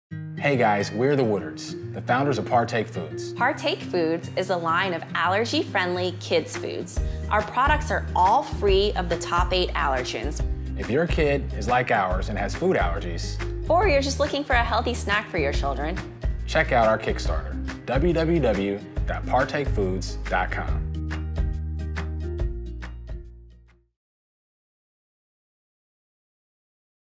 Partake Foods - Coming Soon Teaser
Partake Foods is a line of allergy friendly kid snacks. Make sure to check them out: partakefoods.com